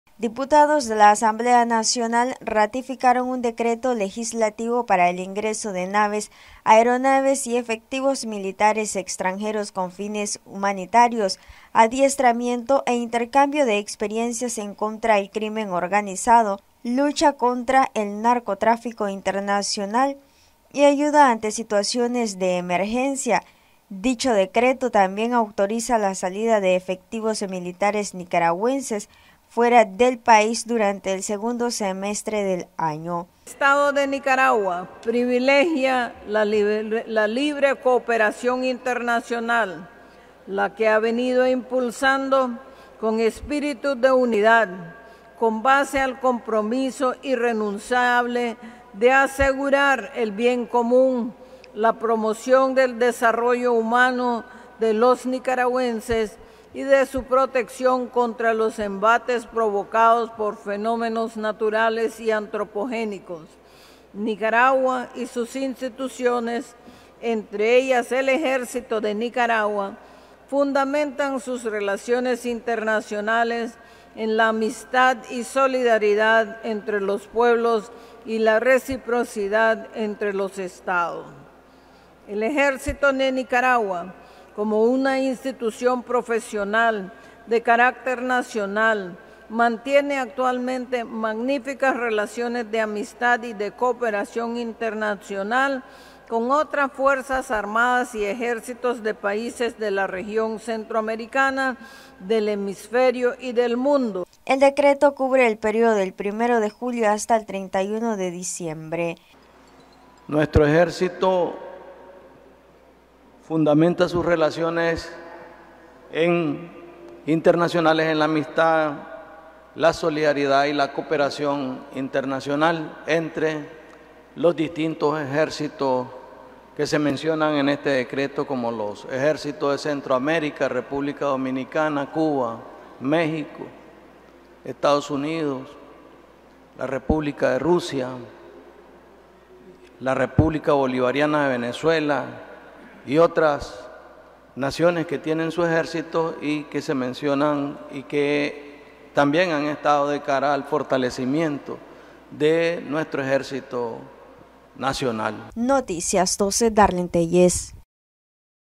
Noticias12Nicaragua Diputados ratifican decreto que autoriza el ingreso de militares extranjeros
Diputados ratifican decreto que autoriza el ingreso de militares extranjeros con fines humanitarios al país